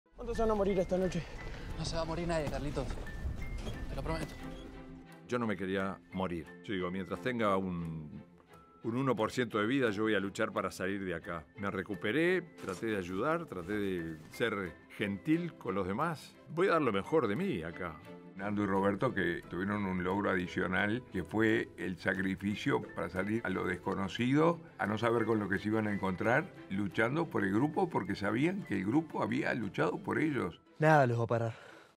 La DESICIÓN de Nando Parrado y Roberto Canessa❄️
Nando Parrado y Roberto Canessa CAMINARON 🚶durante 10 DÍAS en la nieve ❄️ sin saber que si hubieran caminado para el otro lado, se hubieran salvado mucho más rápido ⛰️

El poder del cine 🎬 deja que las personas se acerquen a esta historia y la vivan con, al menos, un mínimo del sentimiento que la vivieron sus protagonistas ✈️

Entérate más de esta y muchas más historias del cine en Very Essence Films 🎞️